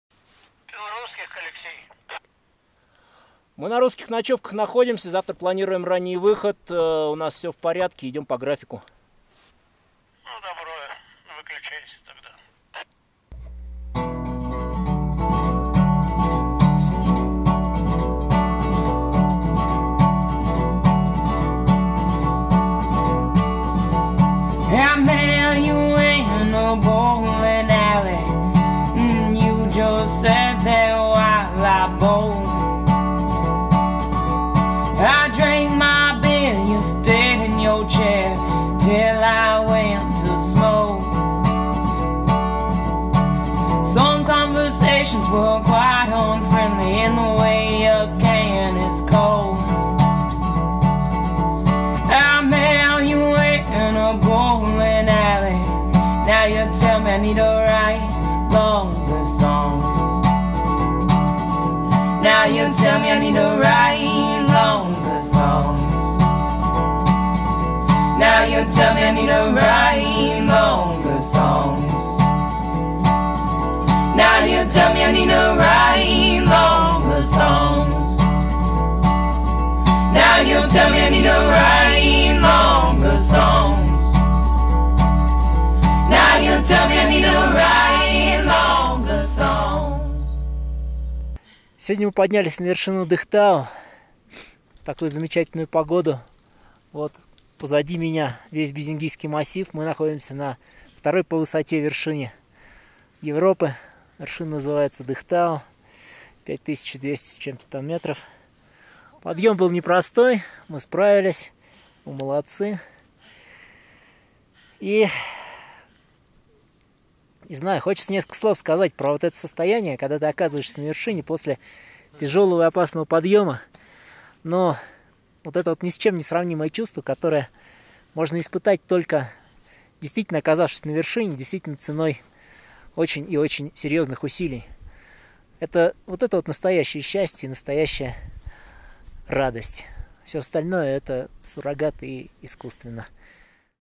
Dykh Tau climb by Alex Trubachev
Short version of the Dykh Tau classic route climb by MCS AlexClimb Team in the season 2018